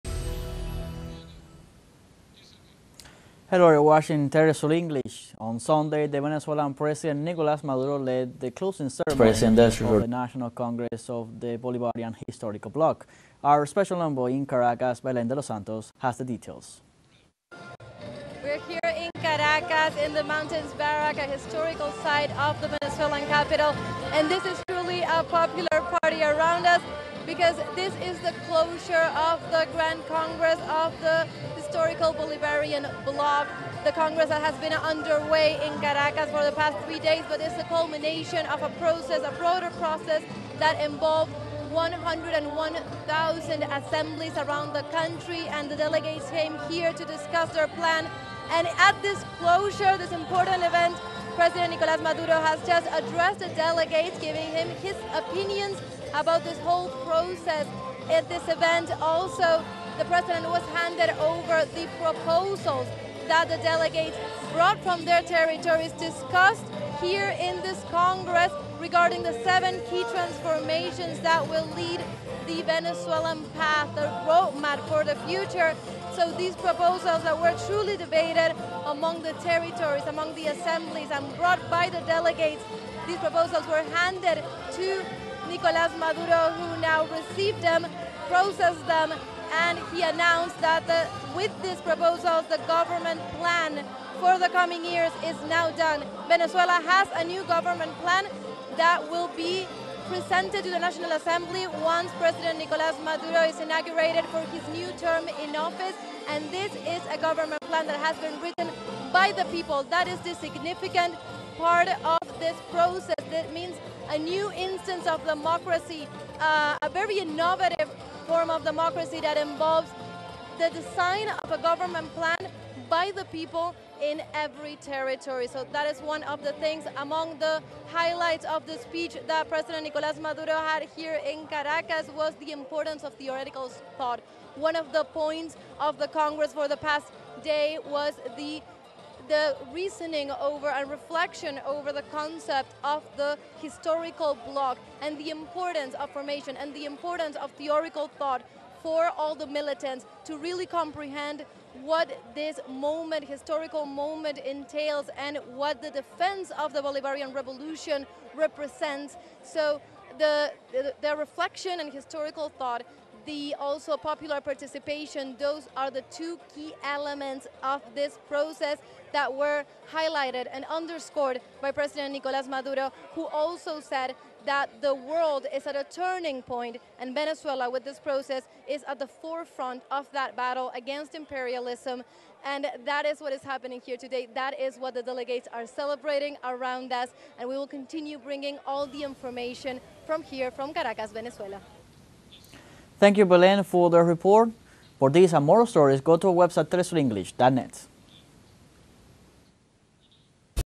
Venezuela | National Bolivarian Congress concludes after four days
TeleSUR Special Envoy Belén de los Santos gives us the details from the culmination of this event, which was held from November 14-17 in Caracas, Venezuela.
teleSUR